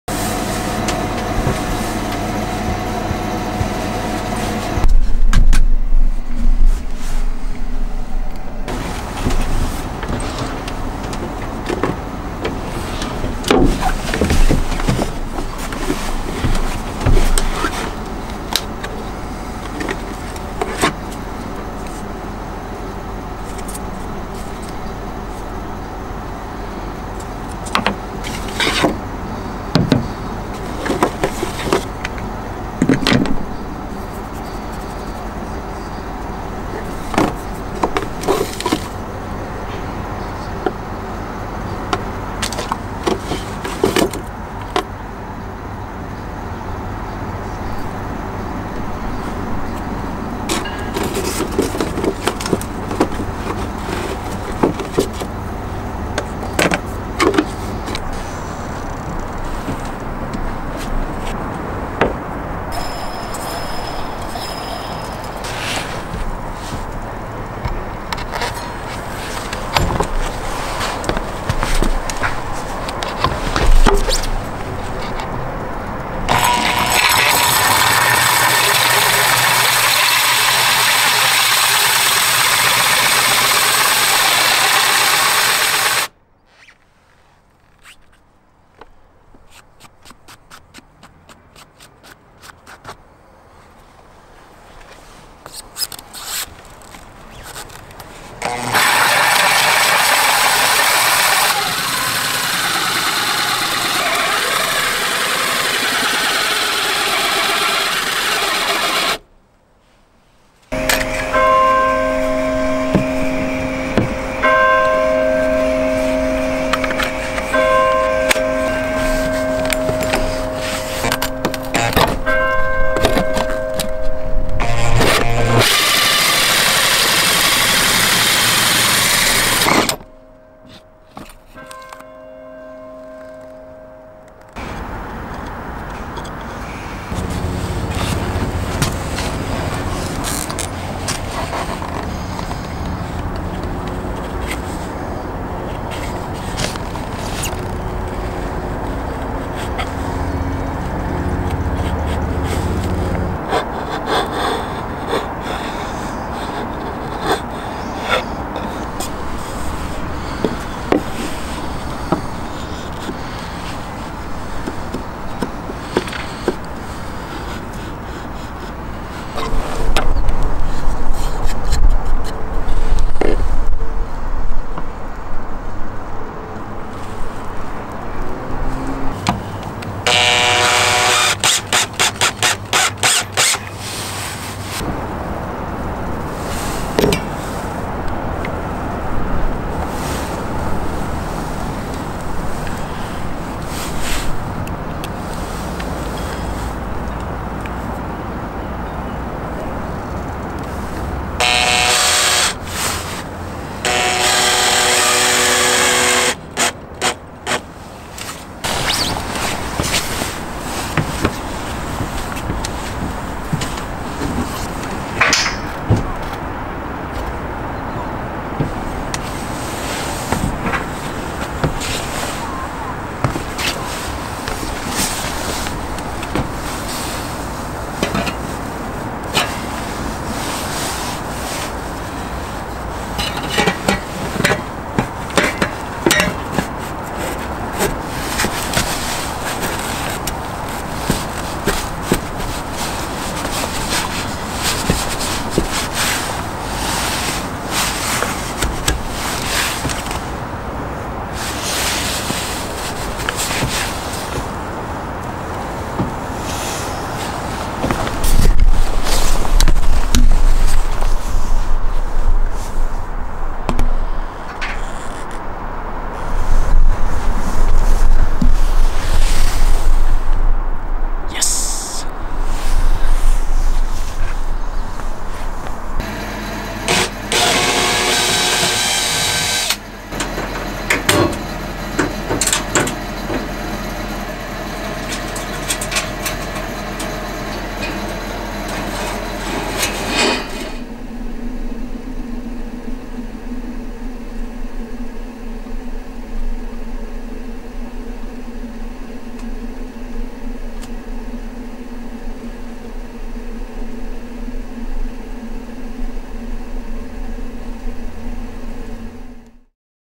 Installing a wood stove // VLOG 6
Finally, after days of pondering and several tool purchases I finally installed the Dickinson Newport Solid Fuel Heater on my Storfidra.